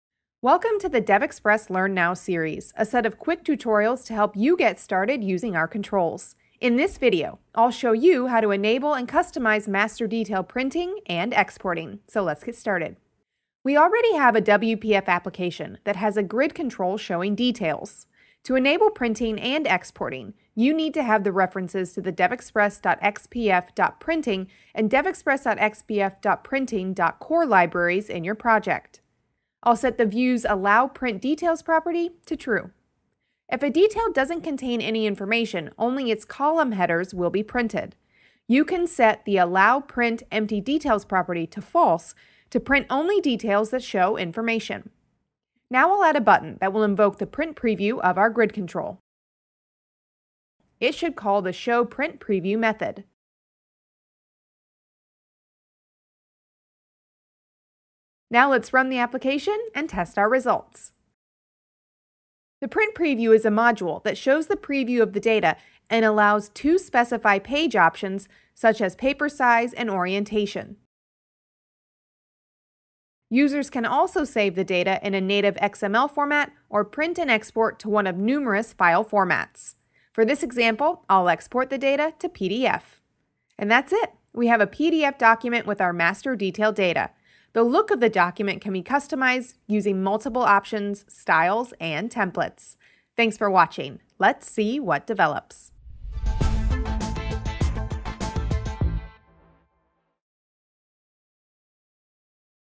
WPF Data Grid - Master-Detail Printing
In this video you'll learn how to include printing for master-detail reports.